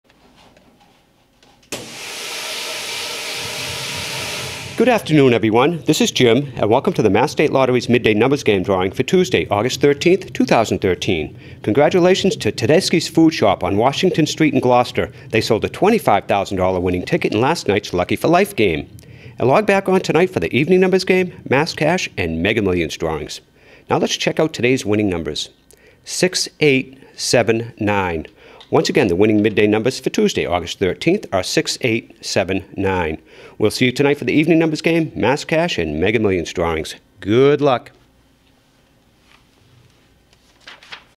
Midday Numbers Game Drawing: Tuesday, August 13, 2013
The official winning numbers in the Mass. State Lottery's Midday Numbers Game drawing for Tuesday, August 13, 2013 are: 6-8-7-9.